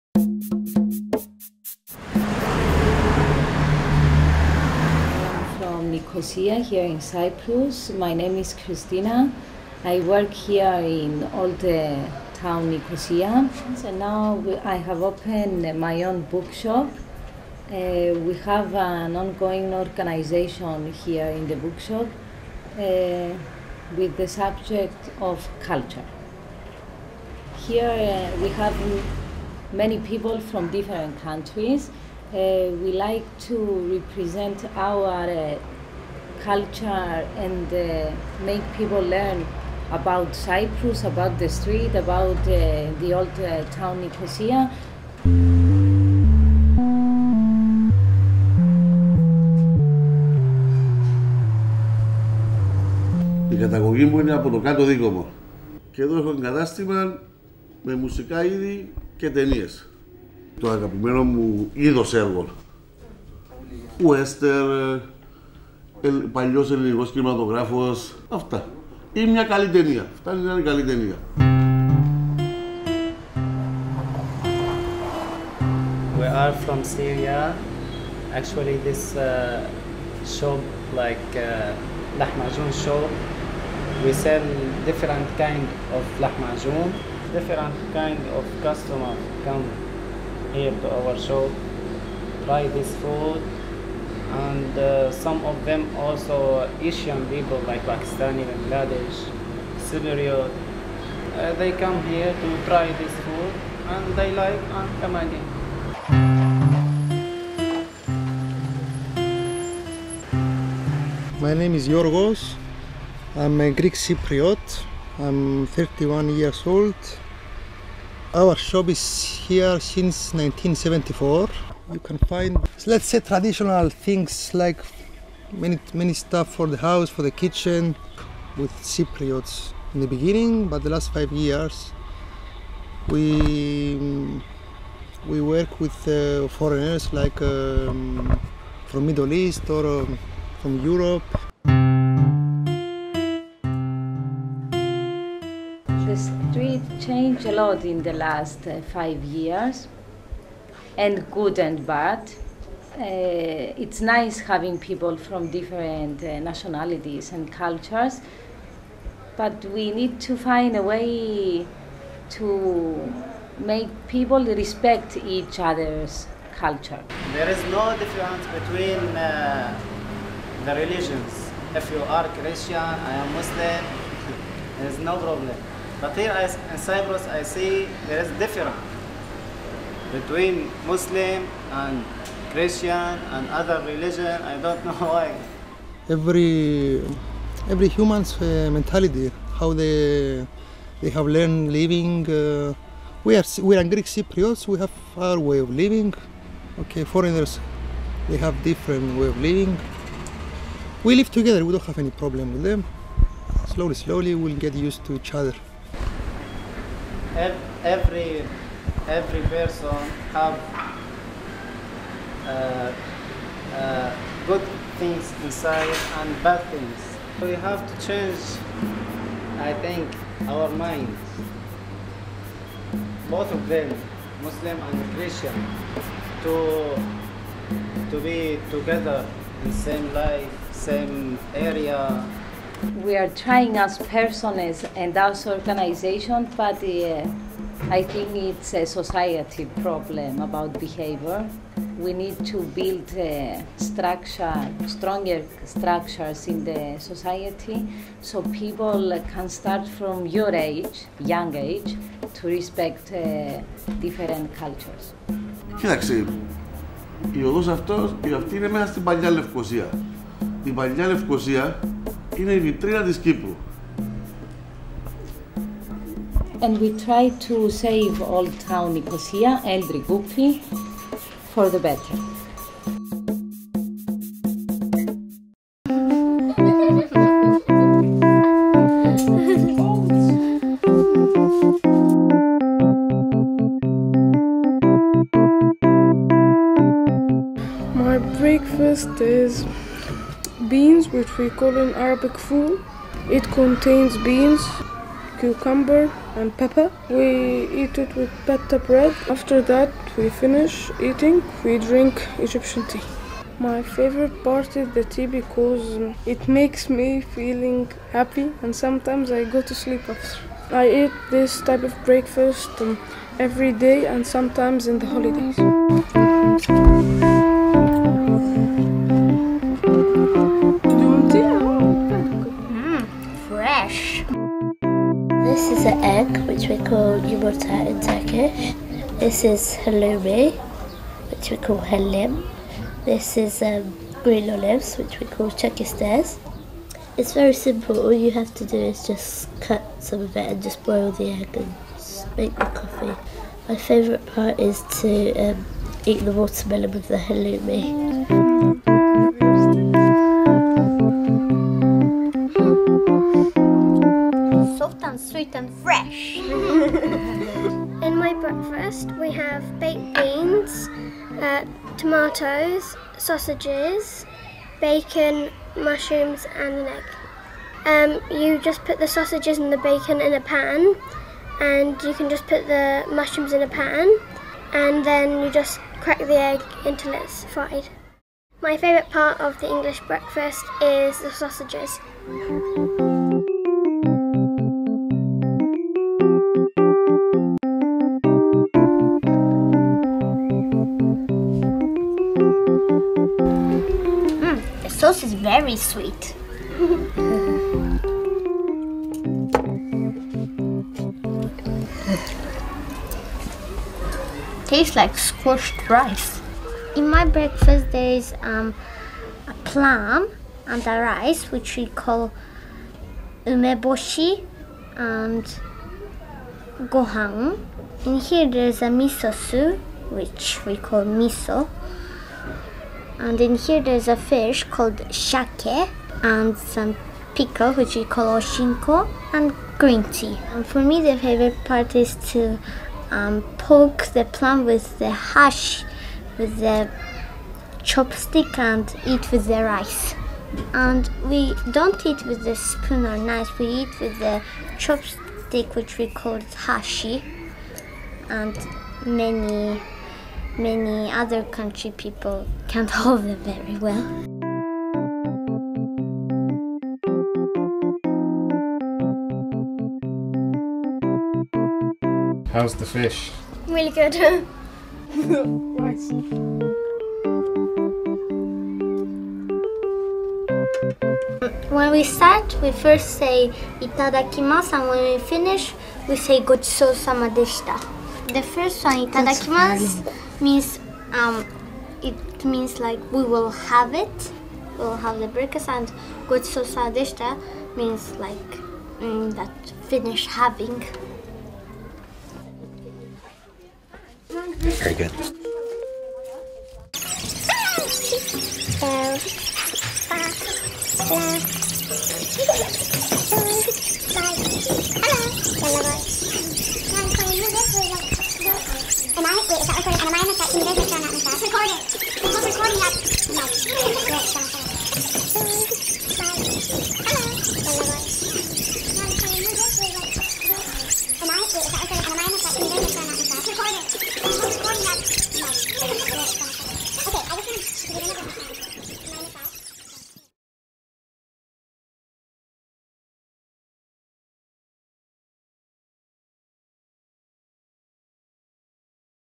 Handful of Culture (Part 2)
Four short films made entirely by Nicosia's Highgate School's 2010 graduating class, with training and a little technical help from CCMC. The film aims to raise awareness about cultural differences and respect for others and gives viewers the chance to see school life through the eyes of the students.

The work was envisaged in the context of Highgate School's Media Studies program with the assistance of the International Children's Film Festival of Cyprus (ICFFCY), production support by CCMC and funding from the Stelios Philanthropic Foundation.